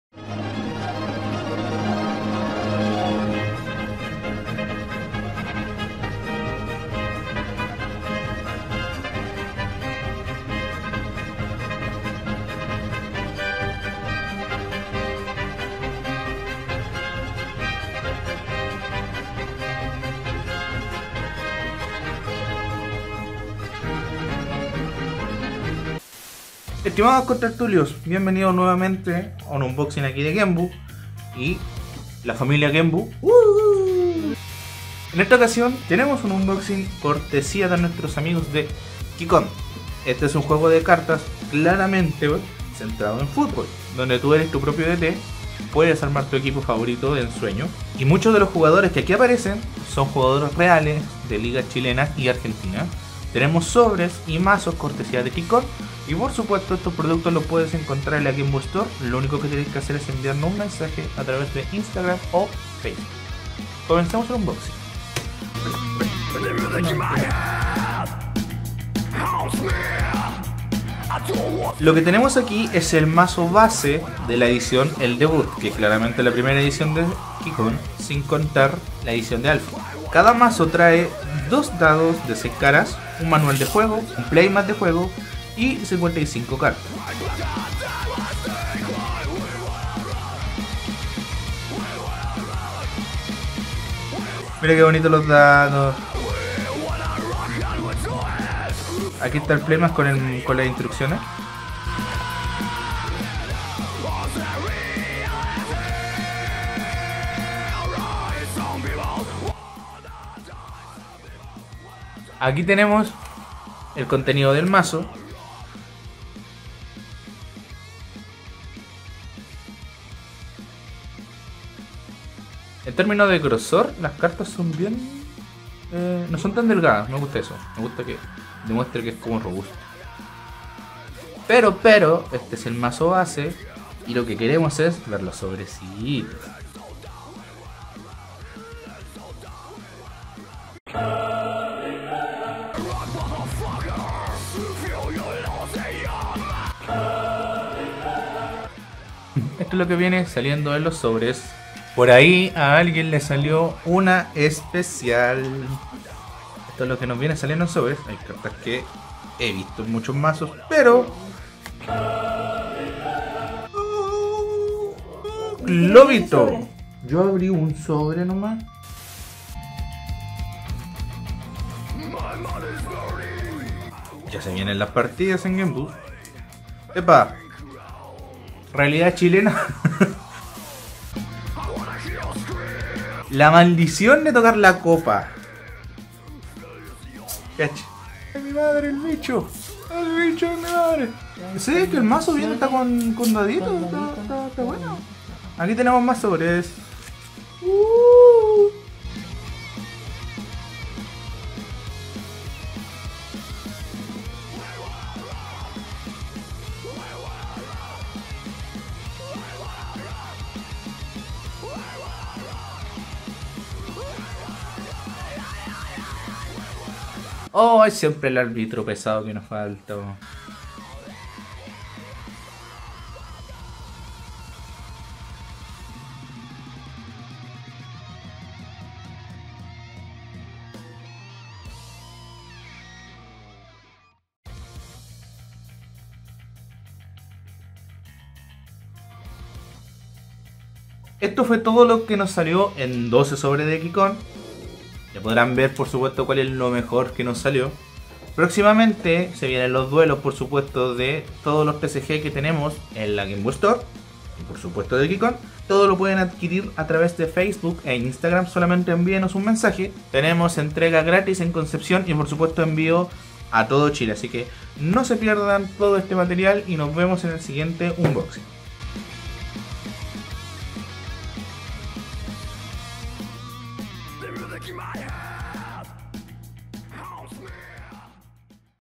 Unboxing: Mazo y Sobres Kick On
Nuestros amigos de Kick On nos enviaron unos regalitos porque saben lo friki que somos en Genbu, y aquí les dejamos lo que fué el unboxing de 12 sobres y 1 mazo de Kick On.